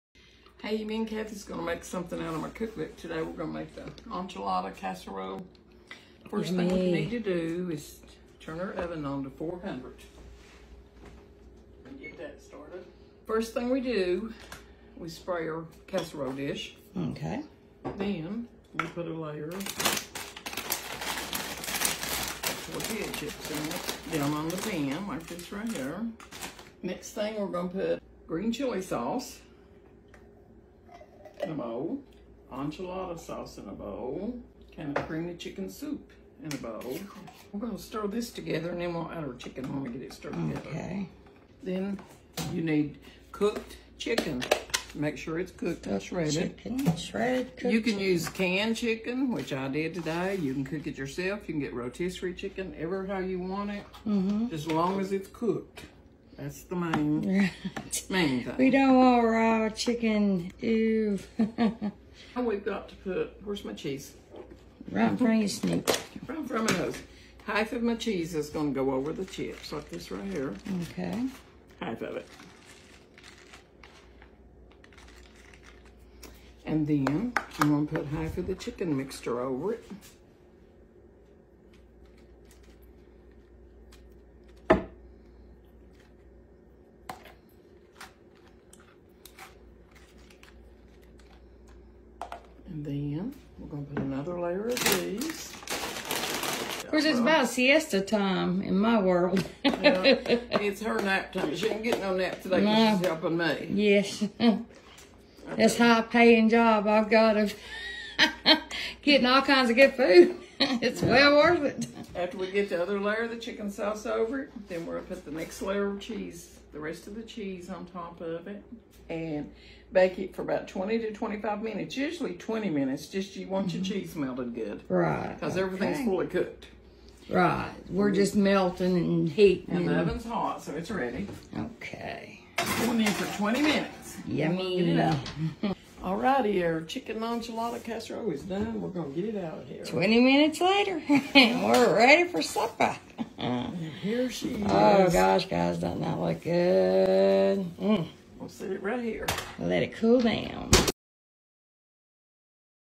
Enchilada Casserole | Dinner ideas | Linda Skeens - Blue Ribbon Kitchen
I love a casserole for an easy dinner when I’ve had a busy day. I love this Enchilada Casserole! It’s full of flavor and comes together so easily. I like to add tomatoes on top when it’s cooled off a little.
What else would you top it with?
Enchilada Casserole
I like that this recipe is simple and doesn't have too many ingredients. It's a great weeknight dinner when you don't have extra time to spend in the kitchen, and it's got flavors everybody loves.
(15-ounce) bag tortilla chips, divided
2 cups shredded mild Cheddar cheese, divided
217 r
1
cups shredded cooked chicken (15-ounce) can green chili sauce (10.5-ounce) can cream of chicken soup
(10-ounce) can enchilada sauce
1. Preheat oven to 400°. Grease a 13x9- inch baking dish.
2. Arrange a layer of chips on the bottom of prepared pan. Sprinkle 1 cup cheese on top.
3. In a medium bowl, stir together chicken, chili sauce, soup, and enchilada sauce. Pour half of the chicken mixture over the cheese layer. Add another layer of chips, and spread remaining chicken mixture on top. Sprinkle with remaining 1 cup cheese.
4. Bake until bubbly and lightly browned, 20-25 minutes.